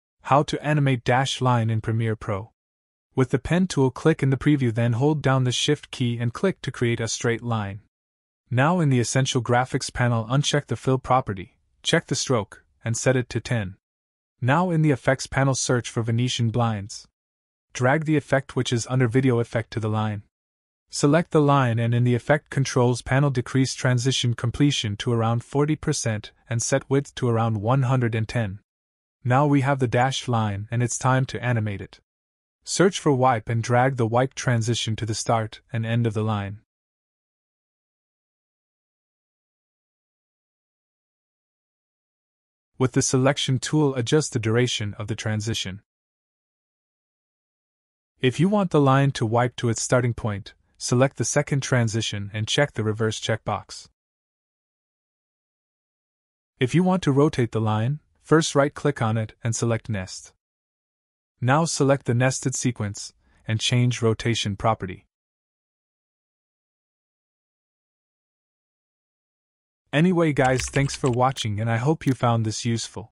Animated DASHED LINE in Premiere Pro
To animate a dashed line in Premiere Pro, create a straight line with the pen tool, set stroke properties, add Venetian blinds effect, adjust parameters for dashed effect, apply wipe transitions for animation, adjust duration, and optionally nest the line for rotation.

"Discover How to Create Animated Dashed Lines in Premiere Pro! In this tutorial, I'll guide you through the process of animating a dashed line using Adobe Premiere Pro. Begin by using the pen tool to create a straight line in the preview window, holding down the shift key for precision. In the essential graphics panel, customize the line by unchecking the fill property, checking the stroke, and setting it to your desired thickness, such as 10. Next, enhance the line's appearance by adding the Venetian blinds effect from the effects panel, adjusting the transition completion and width settings in the effect controls panel to achieve the dashed line effect.

To animate the dashed line, search for and apply the wipe transition to both the start and end of the line. Use the selection tool to adjust the duration of the transition to suit your animation needs. If you want the line to wipe back to its starting point, simply check the reverse checkbox in the transition settings.

For additional customization such as rotating the line, you can nest the line by right-clicking and selecting 'nest.' Then, access the nested sequence to modify properties like rotation for more dynamic animations. Follow these steps to create captivating dashed line animations in Premiere Pro and elevate your video editing skills!"

How to Create Animated DASHED LINE in Premiere Pro Chapters:
00:00 draw a line
00:14 use venetian blinds
00:31 use wipe transition
00:55 rotate dashed line